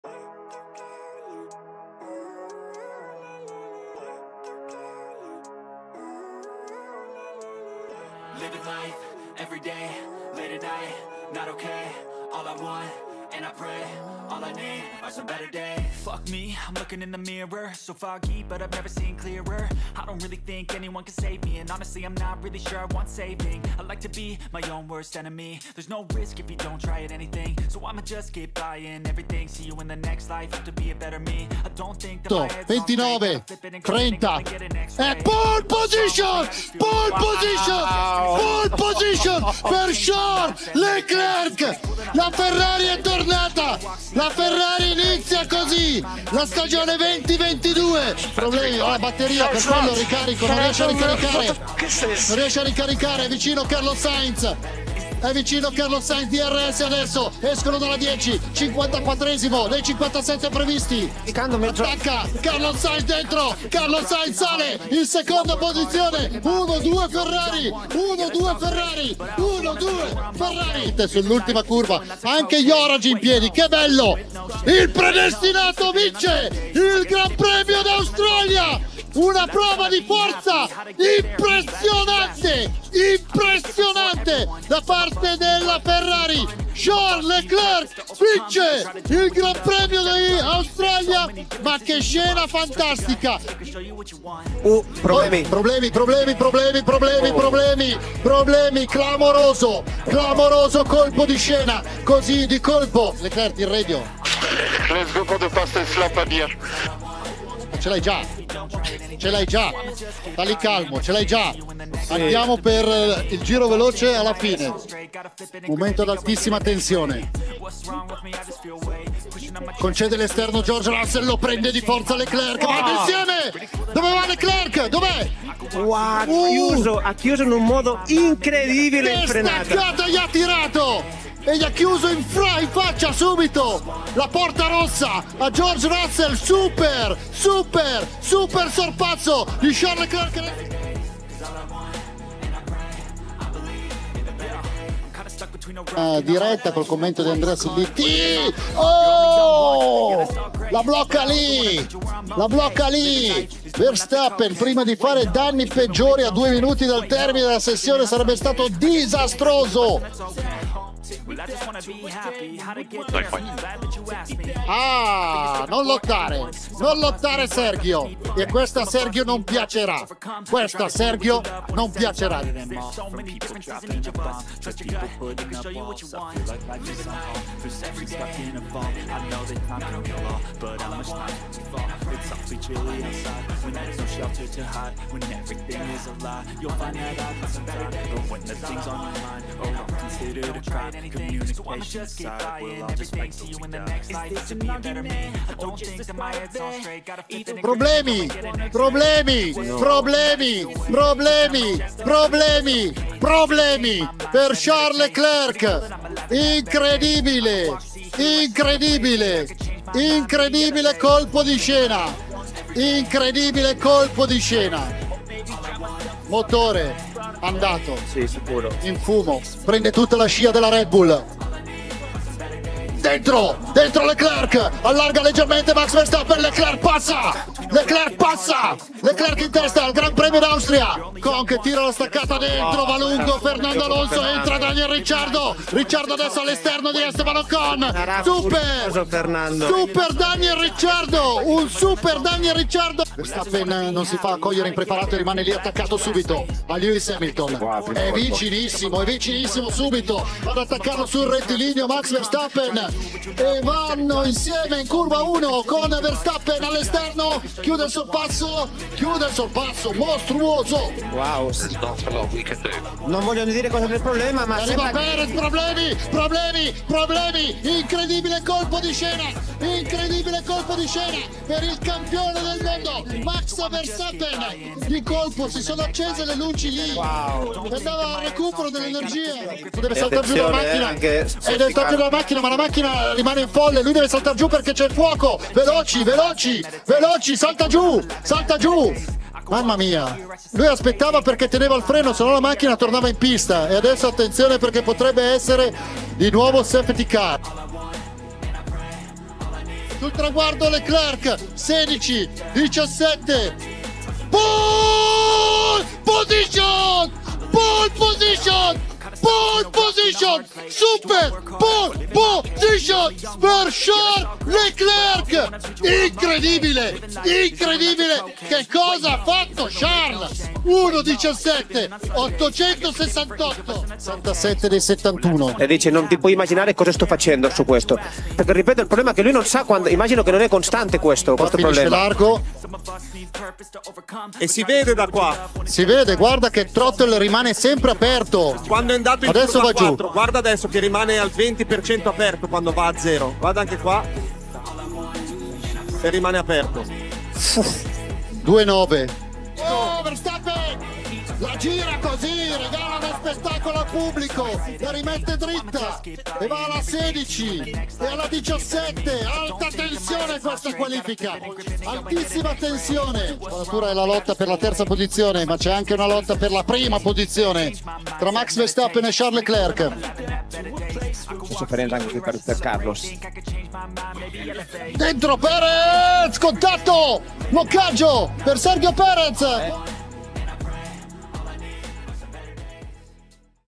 BEST of CARLO VANZINI |Formula Uno| parte 1
Mi sono avvicinato a questo sport grazie a questo SUPER TELECRONISTA, che con la sua voce,  riesce a farti restare incollato alla tv anche quando la gara non è una delle più emozionanti.

Questa capacità deriva dalla sua immensa passione ed è grazie a Carlo Vanzini e a tutti i suoi colleghi come: Marc Gené, Matteo Bobbi (freebobbi), Davide Valsecchi (freevalse), Roberto Chinchero, Federica Masolin e Mara Sangiorgio che stanno contribuendo a far conoscere la Formula 1 in Italia.

Se volete una seconda parte fatemelo sapere nei commenti (mancano molte clip da brividi)😅

Ah sempre e comunque FORZA FERRARI
✔ Grazie a tutti ヅ ヅ